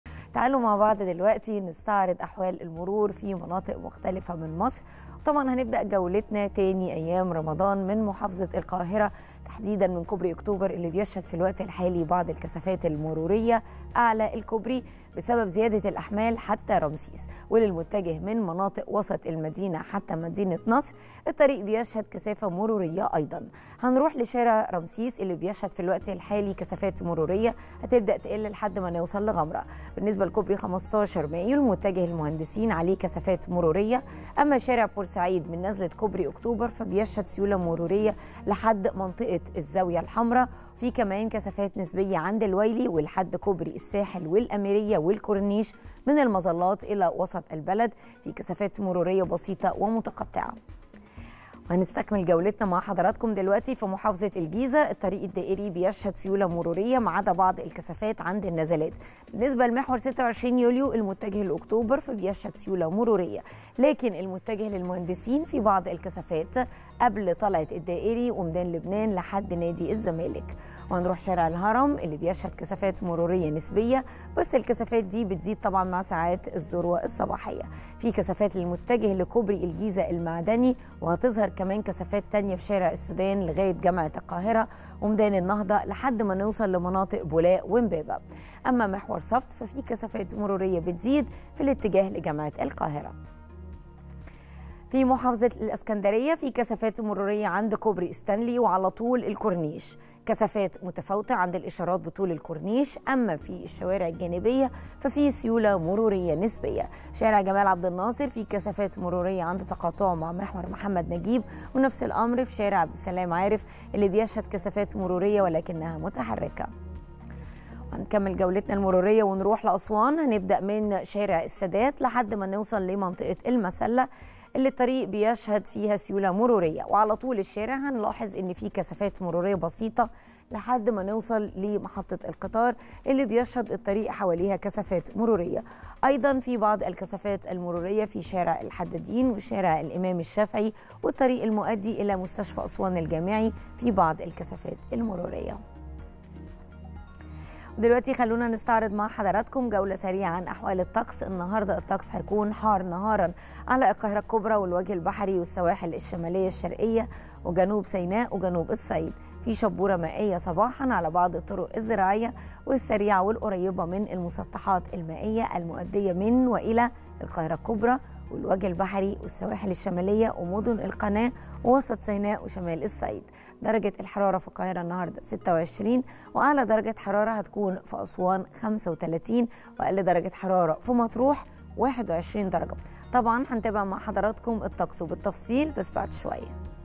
تعرف على الحالة المرورية بشوارع القاهرة والجيزة في صباح تاني أيام رمضان
صباح الخير يا مصر | تعرف على الحالة المرورية بشوارع القاهرة والجيزة في صباح تاني أيام رمضان